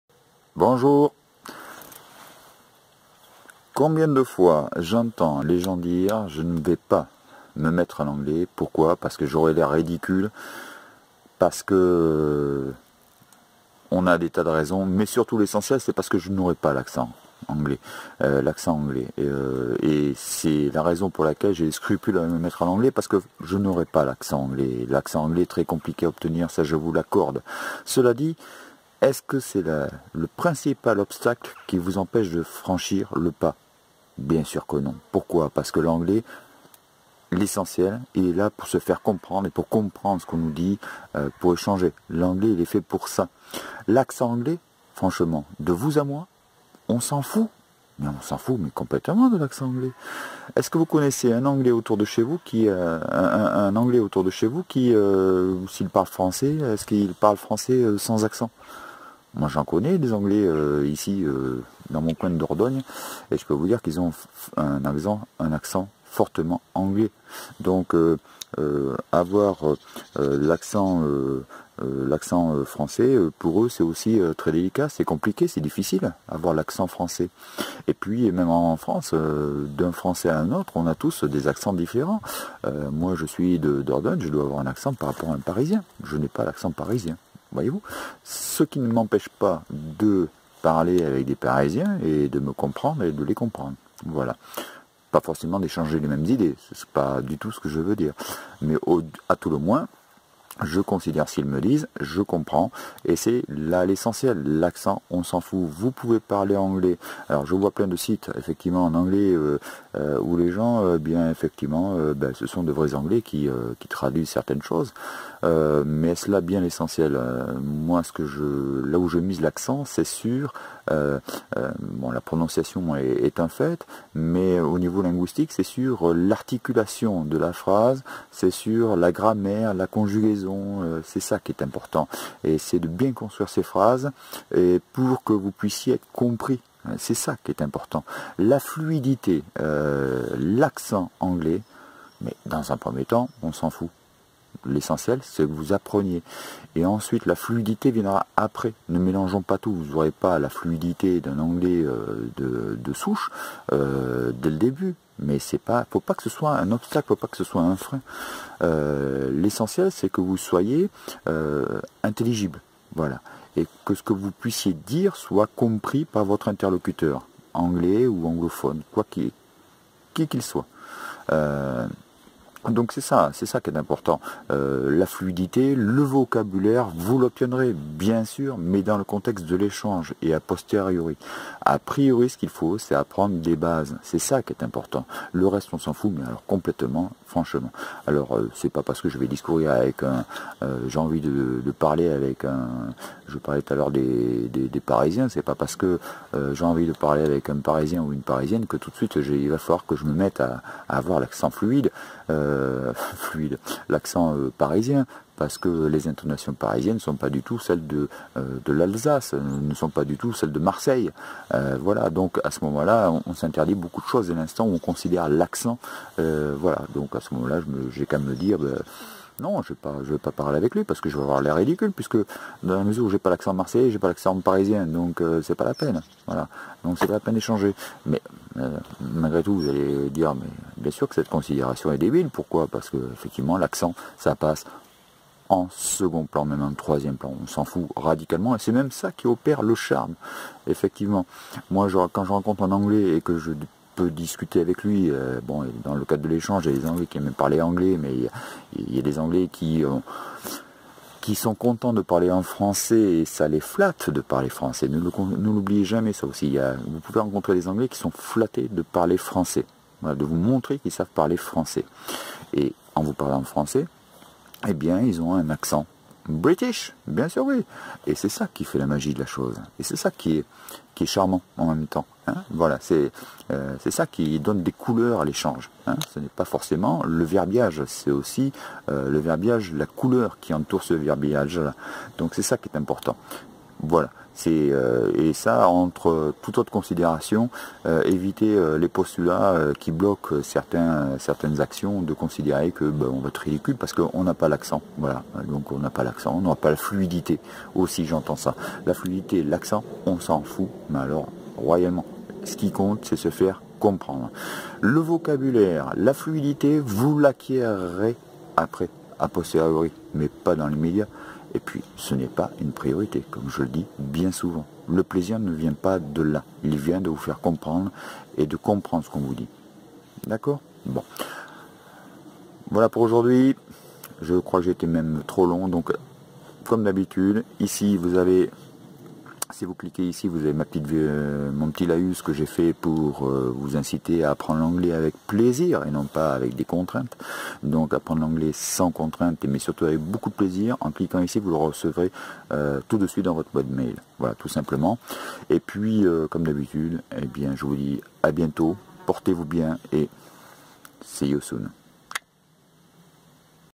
Accent Français quand on parle Anglais, c'est important ?
Dans cette vidéo, je vais vous soumettre la question de l'accent anglais quand on souhaite parler avec un anglais. Est-ce vraiment important ?